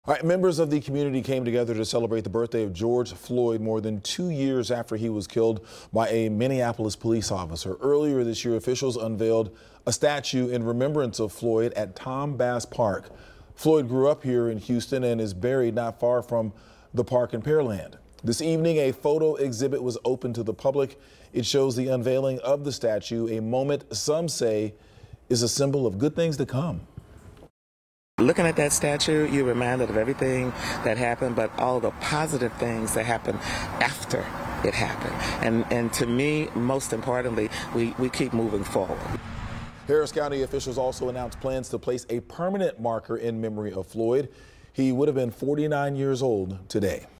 Community remembers George Floyd on his 49th birthday with special celebration, photo exhibit
Members of the community came together to celebrate the birthday of George Floyd more than two years after he was killed by a Minneapolis police officer.